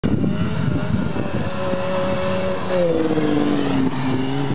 1999 Yamaha Phazer 500 jump
Jordan "phazerboy" LeMay jumpin the old phazer near The Pas, MB